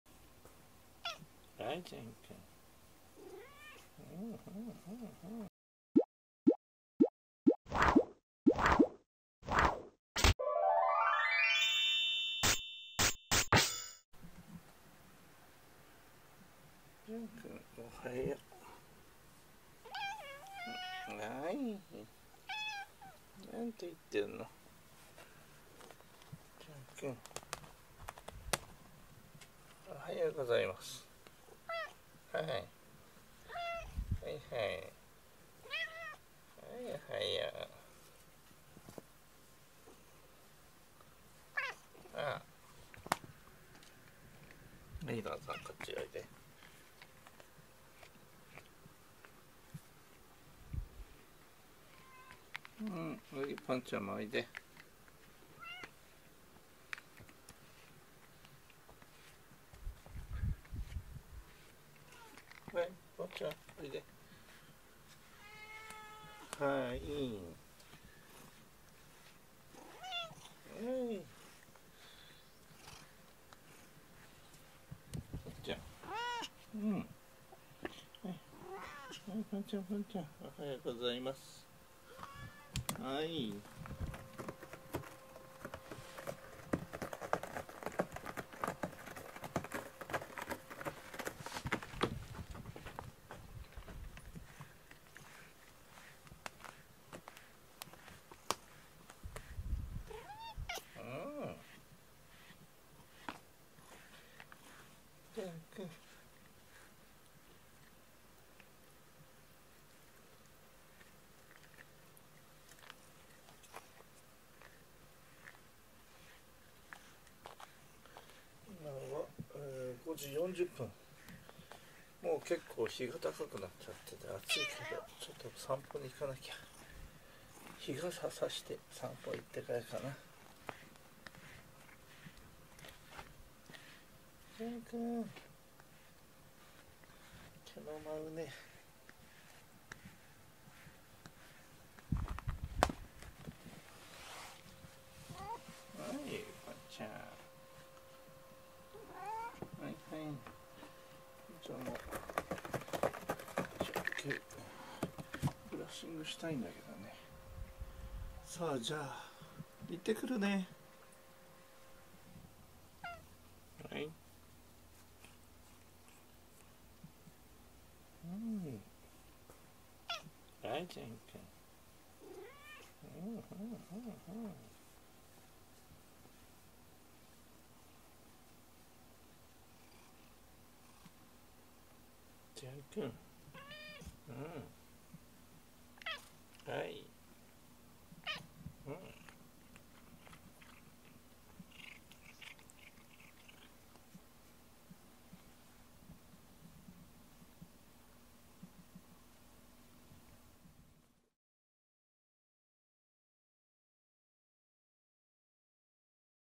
朝から晩まで撫で回される猫ジャンくんとポンちゃん【Jean & Pont 3268】2023/7/12 保護猫ジャンけんポン
パパとママはジャンくんとポンちゃんのことが大好きです。
だから近寄ってきたら必ず撫でます。
この２匹の猫たちもよくわかっていて「ここでよろしく」って撫でポイントに来て待ちます。
相思相愛の仲ということです。